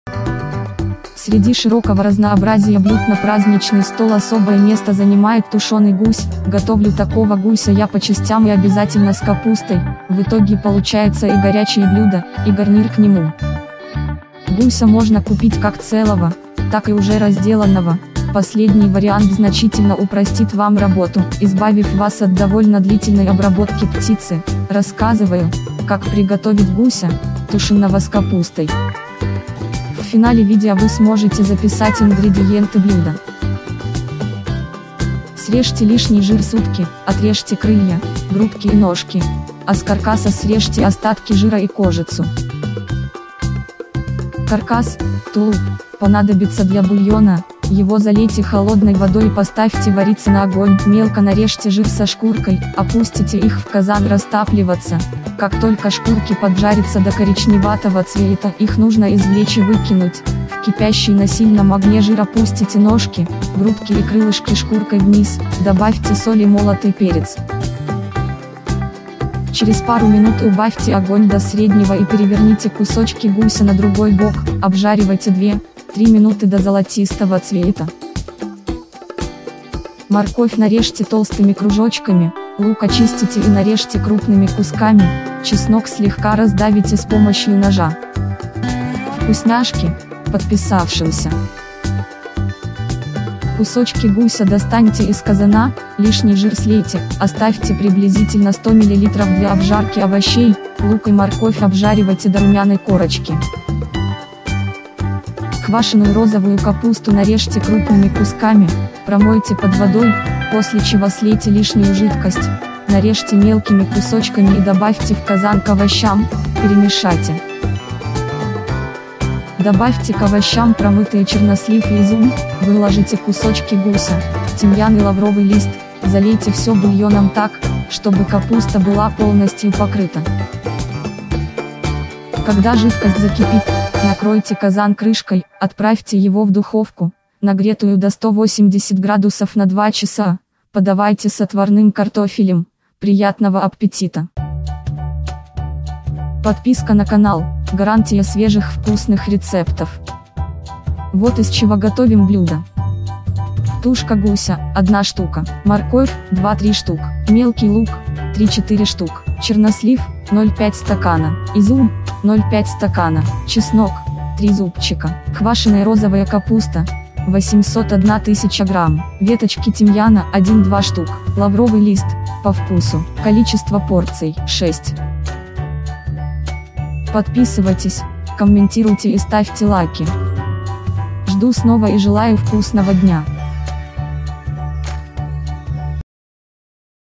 ★КАК У БАБУШКИ.★ ГУСЬ, ТУШЕННЫЙ С КАПУСТОЙ ★
★Вкуснятинка! Среди широкого разнообразия блюд на праздничный стол особое место занимает тушеный гусь● Готовлю такого гуся я по частям и обязательно с капустой● В итоге получается и горячее блюдо, и гарнир к нему●